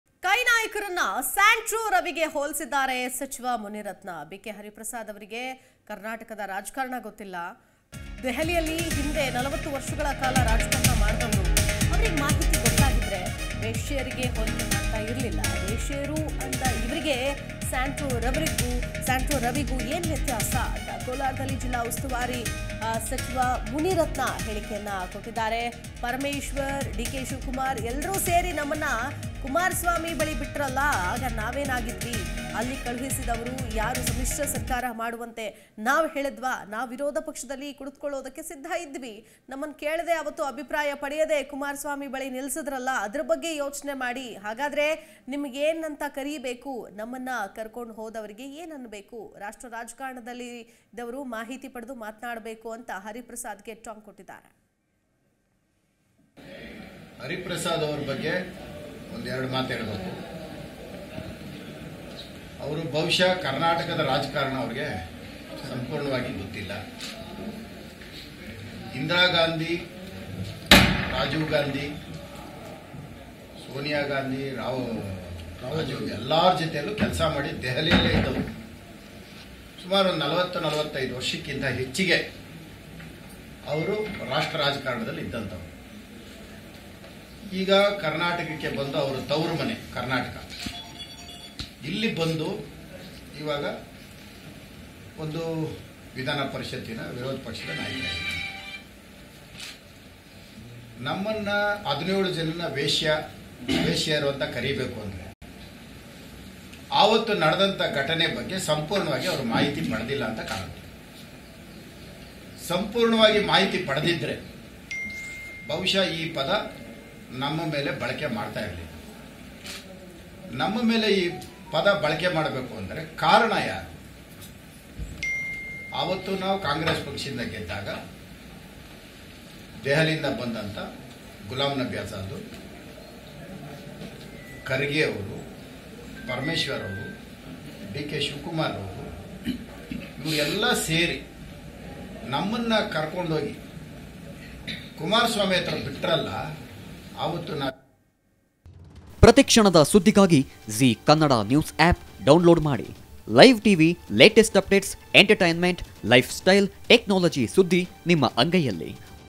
Muniratna | Santro Ravi | ಹರಿಪ್ರಸಾದ್‌ಗೆ ಕರ್ನಾಟಕದ ರಾಜಕಾರಣ ಗೊತ್ತಿಲ್ಲ
ಸಚಿವ‌ ಮುನಿರತ್ನ ʻಕೈʼ ನಾಯಕರನ್ನು‌ ಸ್ಯಾಂಟ್ರೋ ರವಿಗೆ ಹೋಲಿಸಿದ್ದಾರೆ. ಬಿ.ಕೆ. ಹರಿಪ್ರಸಾದ್ ಅವರಿಗೆ ಕರ್ನಾಟಕದ ರಾಜಕಾರಣ ಗೊತ್ತಿಲ್ಲ. ದೆಹಲಿಯಲ್ಲಿ ಹಿಂದೆ 40 ವರ್ಷಗಳ ಕಾಲ ರಾಜಕಾರಣ ಮಾಡಿದವರು. ಅವರಿಗೆ ಮಾಹಿತಿ ಗೊತ್ತಾಗಿದ್ರೆ ವೇಶ್ಯೆರಿಗೆ ಹೋಲಿಕೆ ಮಾಡುತ್ತಿರಲಿಲ್ಲ ಎಂದಿದ್ದಾರೆ..